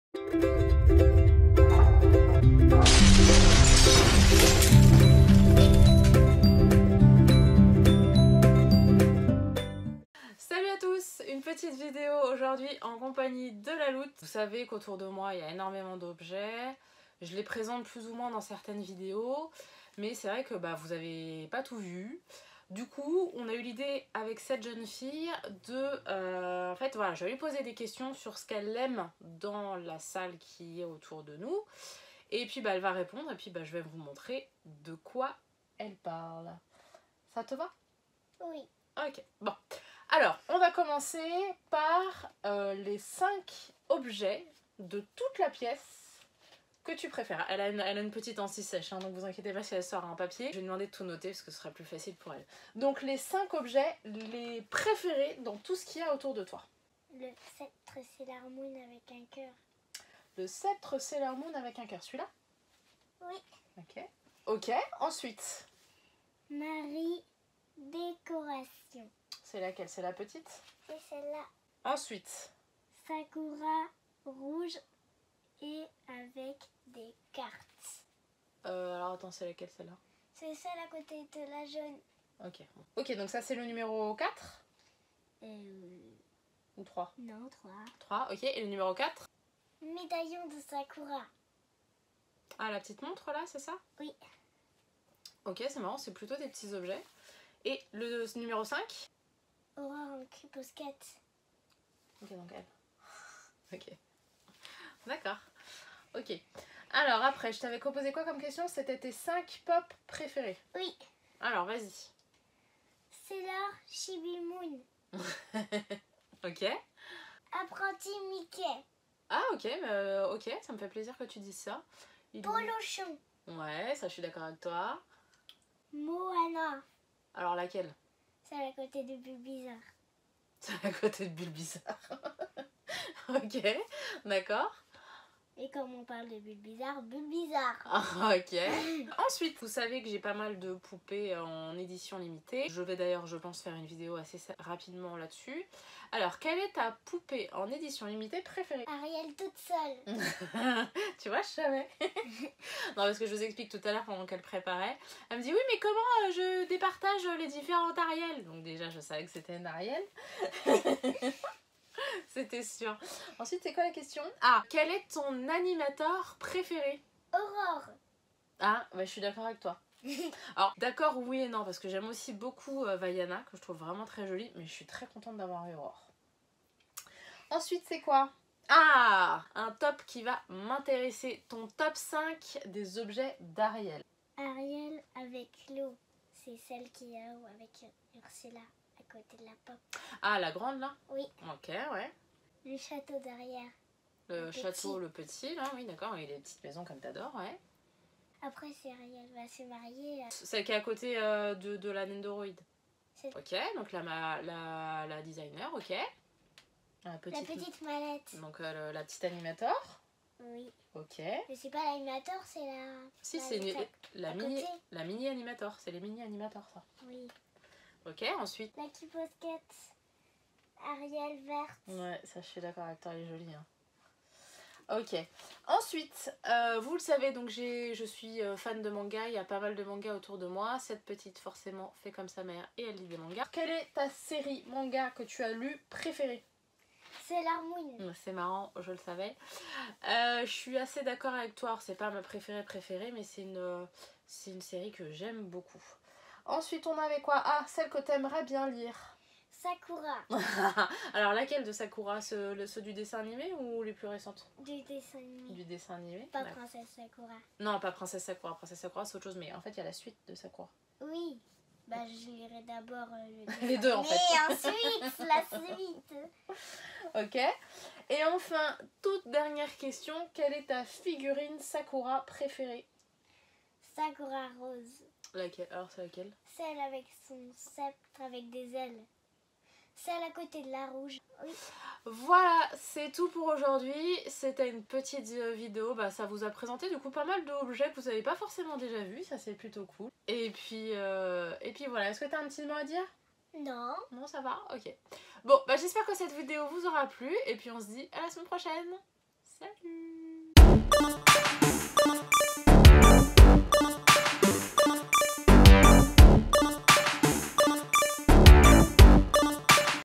COLLECTION | La loute vous présente ses favs !
Pour dérouler, clique sur "PLUS" !

Aujourd'hui, je suis en compagnie de ma fille, la Loute, qui va vous montrer les éléments qu'elle préfère dans ma salle mangas !
J'espère que la vidéo vous plaira ! Et si ce n'est pas le cas, n'hésitez pas à me faire vous retours !

N'hésitez pas à liker, à partager et à commenter !